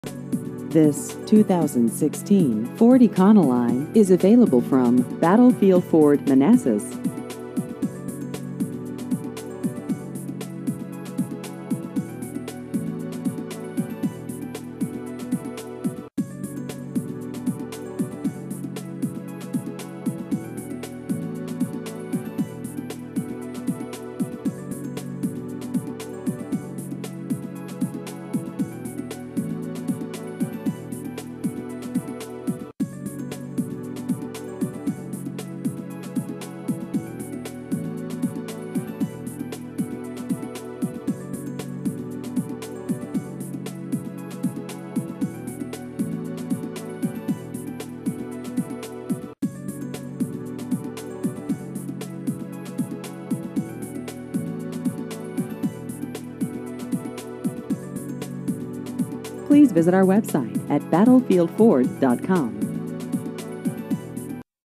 2016 Ford Econoline Commercial Cutaway New Cars Manassas VA
This 2016 Ford Econoline Commercial Cutaway is available from Battlefield Ford Manassas.